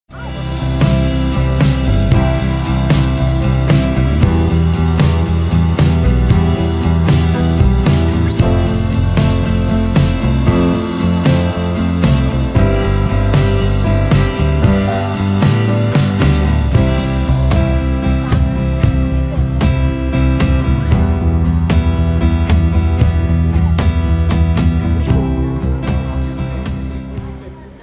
hippopotamus swims in a zoo in Poland
Beautiful hippopotamus eats leaves in the park))

(Hippopótamus amphíbius)